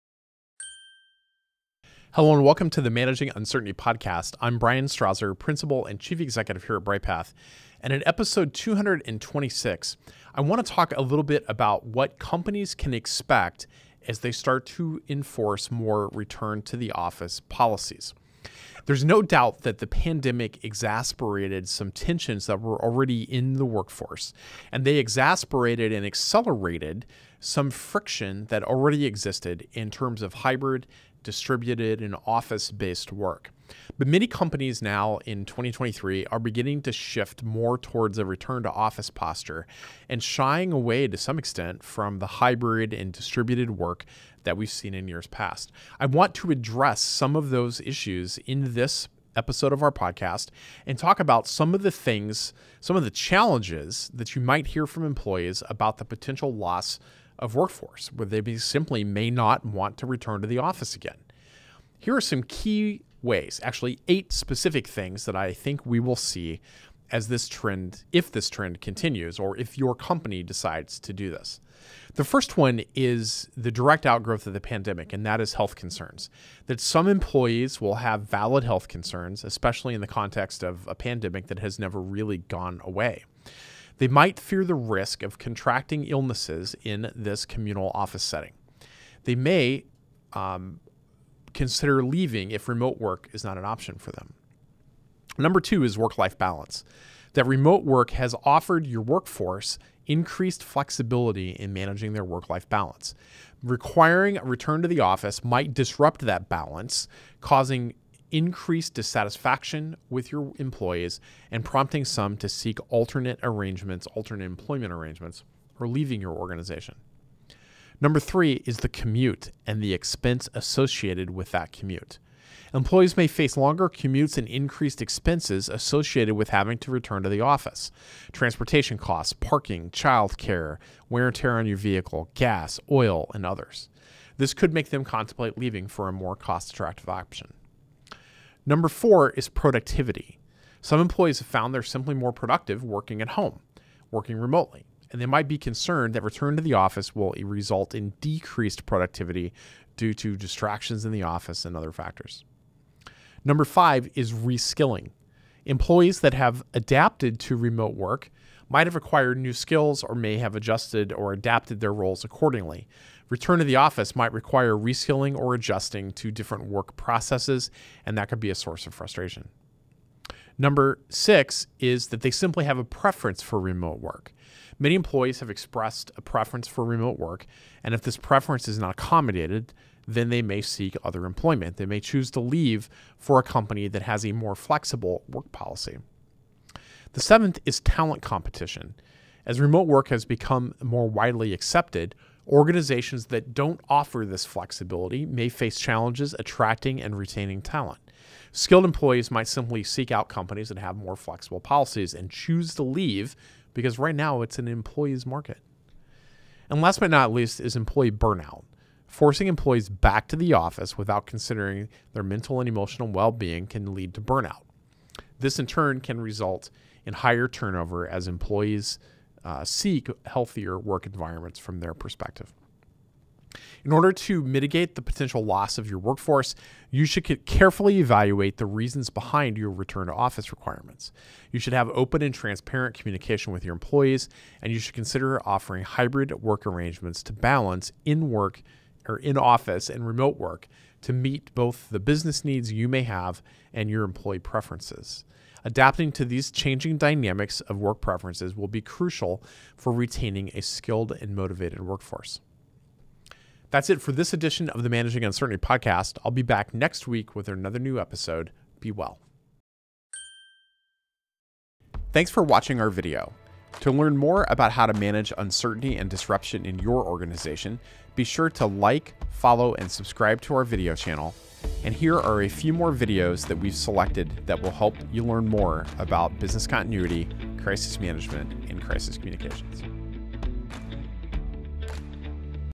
Episode #226 - How will return to the office impact your workforce?
In this episode of Managing Uncertainty, Bryan Strawser, Principal and Chief Executive at Bryghtpath, deep dives into the potential challenges organizations may face as the pendulum swings back towards in-office work in 2023. Bryan discusses eight key obstacles, including health risks, disrupted work-life balance, financial strain of commuting, and the risk of employee burnout that could lead to a loss of workforce.

He further explores how this shift could stifle productivity, necessitate reskilling, and potentially hamper your talent acquisition and retention strategy. However, it's not all doom and gloom. Bryan also offers valuable strategies to help organizations strike a balance between their needs and employee preferences. He emphasizes the importance of open communication, flexibility, and adaptation in this rapidly evolving employment landscape.

Whether you're an employer or an employee, tune in to this enlightening episode and equip yourself with strategies to not just survive, but thrive in these challenging times. Remember, the key to navigating this transition successfully is being prepared.